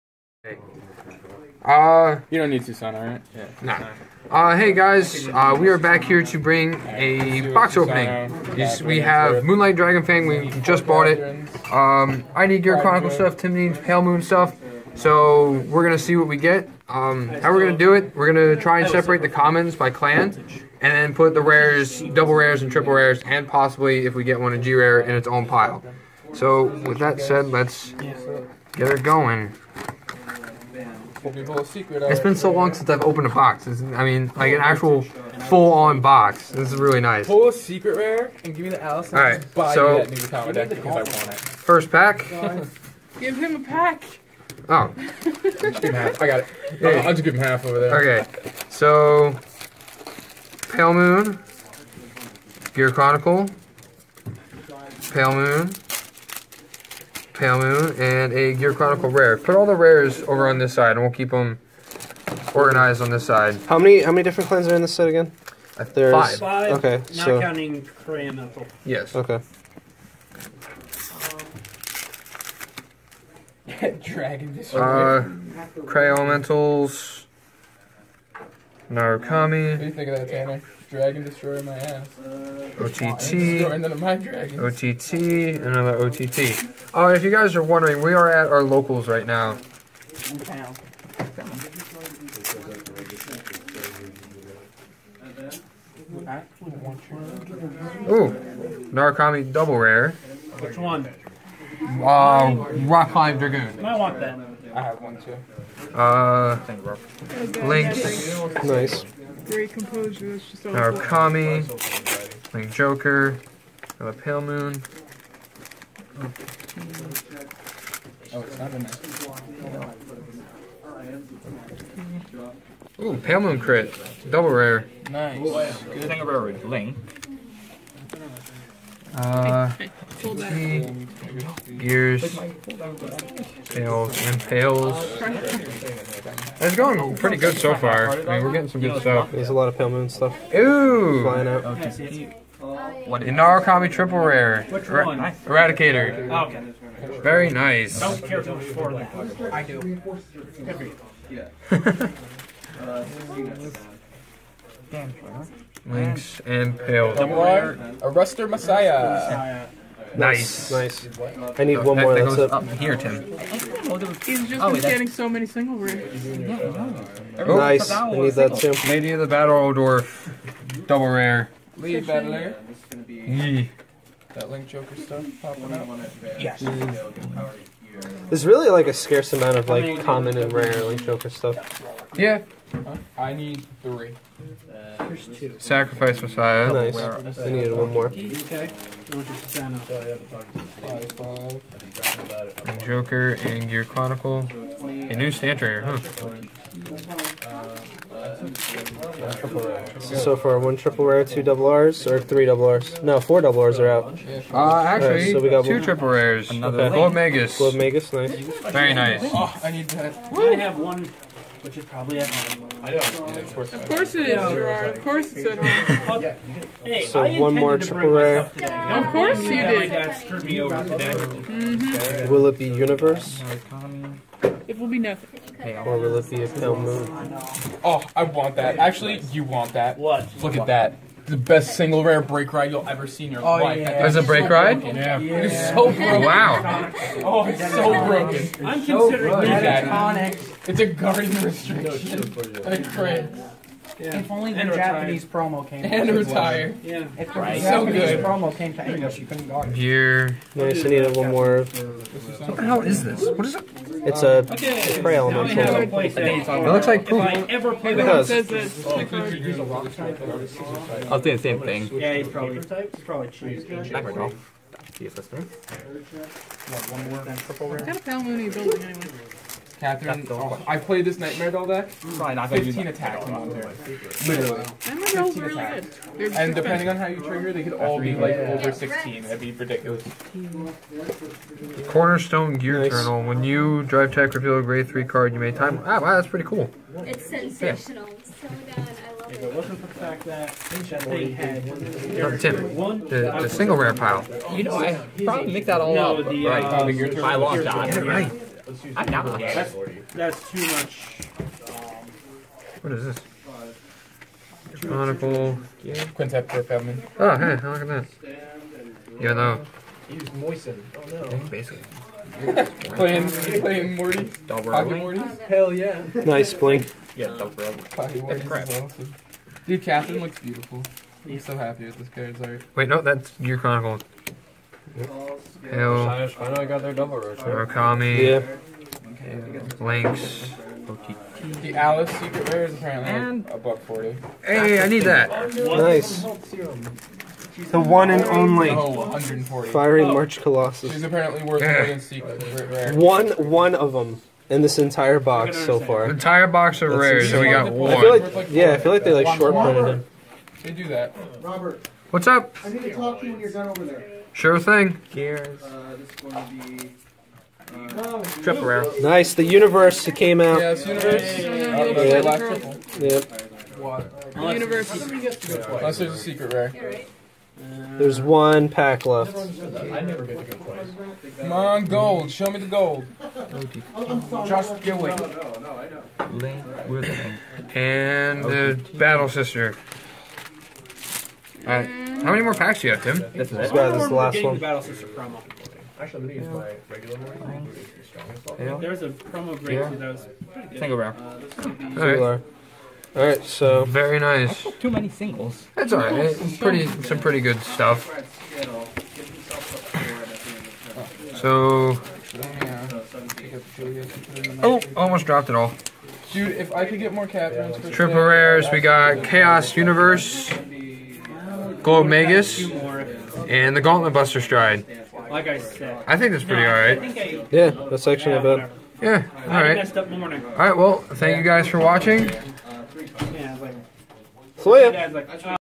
G Set 5 Opening
Well we got some nice pulls but not lucky enough for my pale moons to get a GR...
Also sorry for the background noise, we decided to open the box at our usual card shop.